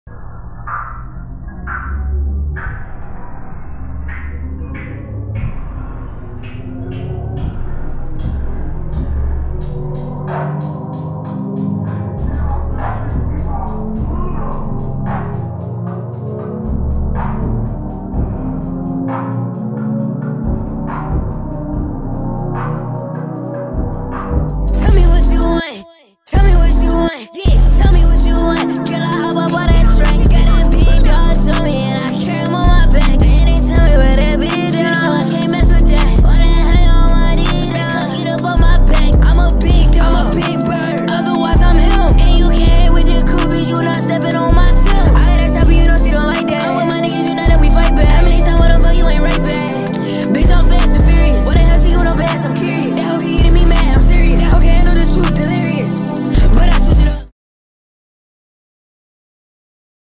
tell me what you want
this my lil brother rapping